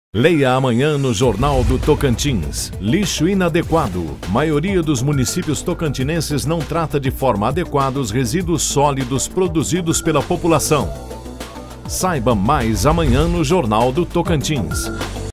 Leia amanhã no Jornal do Tocantins
Lixo inadequado

Maioria dos municípios tocantinenses não trata de forma adequada os resíduos sólidos produzidos pela população